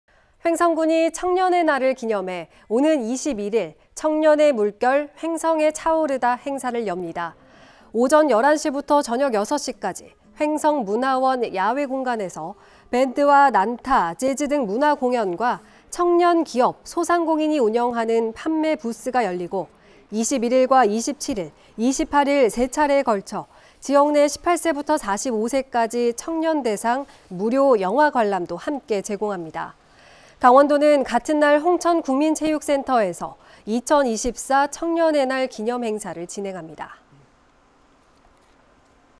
2024. 9. 19 [원주MBC] 횡성군, 21일 청년의날 기념 행사
횡성군이 청년의 날을 기념해 오는 21일
'청년의 물결, 횡성에 차오르다' 행사를
엽니다.

오전 11시부터 저녁 6시까지 횡성문화원
야외공간에서 밴드와 난타, 재즈 등 문화공연과
청년 기업·소상공인이 운영하는 판매 부스가
열리고,

21일과 27일, 28일 세 차례에 걸쳐
지역 내 18세부터 45세까지 청년 대상
무료 영화 관람도 함께 제공합니다.

강원도는 같은 날 홍천 국민체육센터에서
2024 청년의 날 기념행사를 진행합니다.